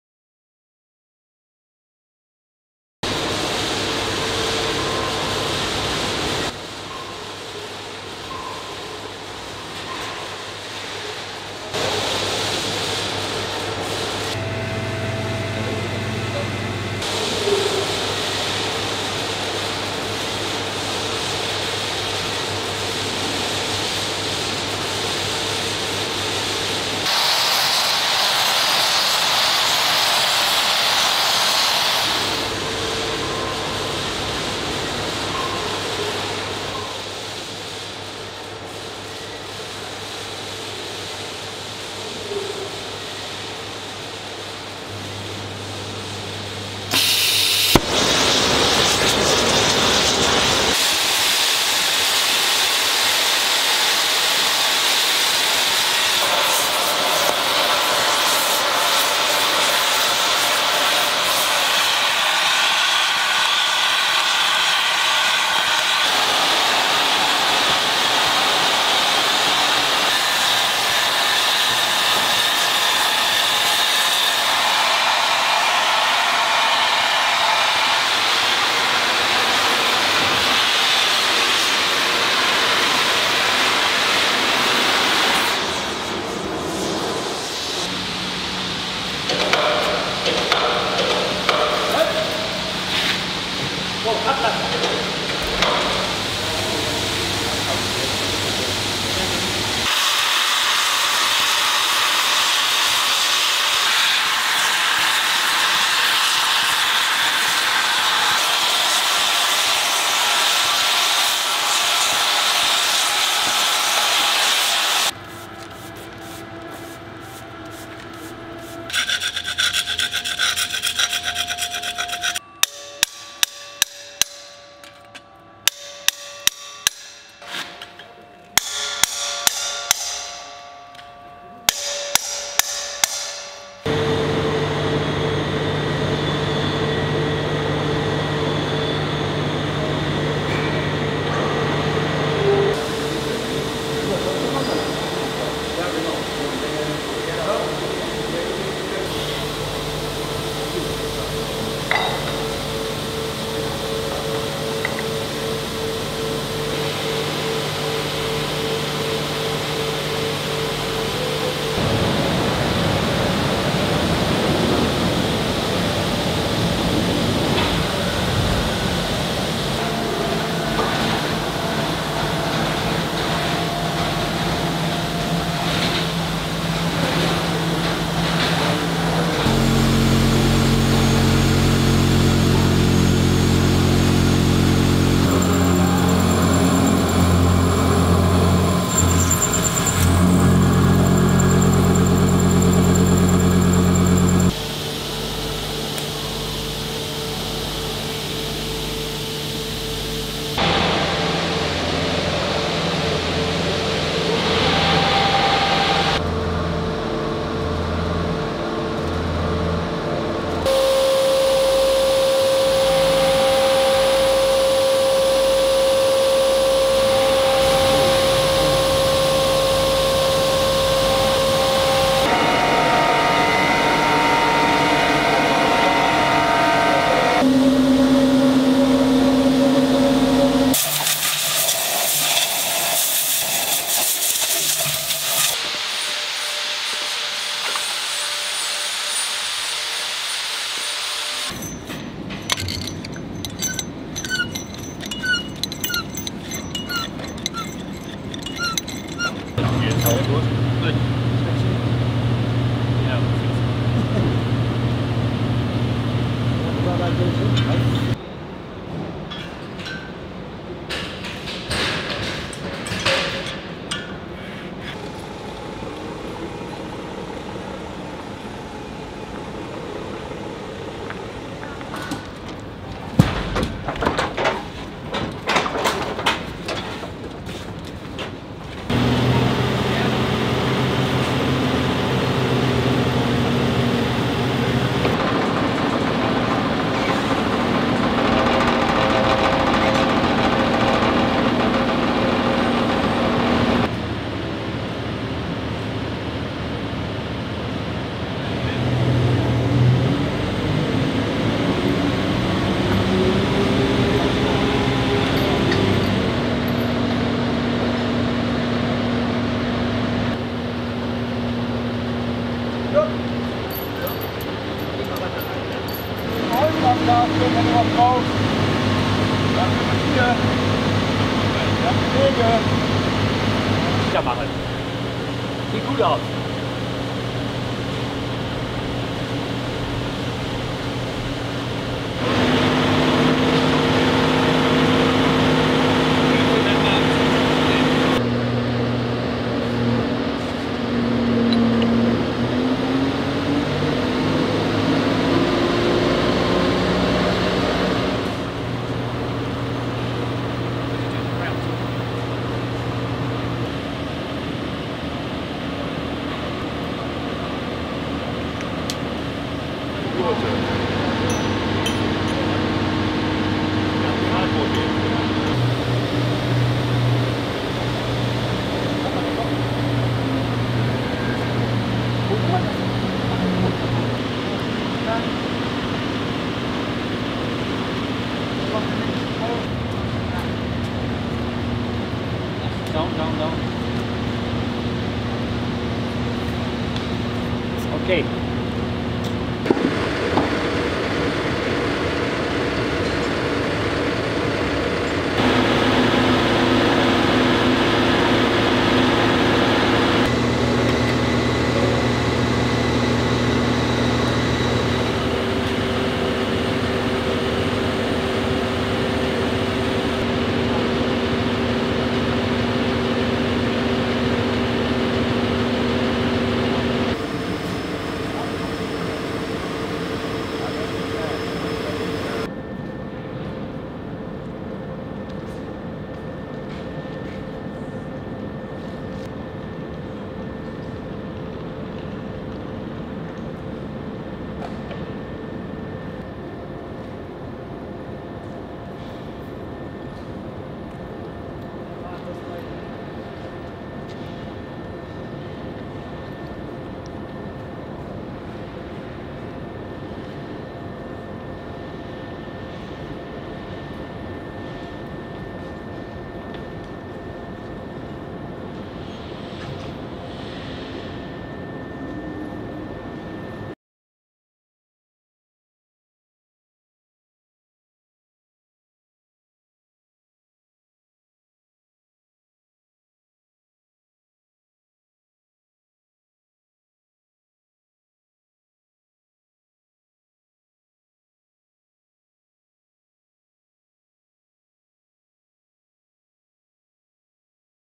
The making of Antony Gormley’s ALERT
Watch how Antony Gormley’s ALERT came together before it was unveiled at our South Kensington Campus.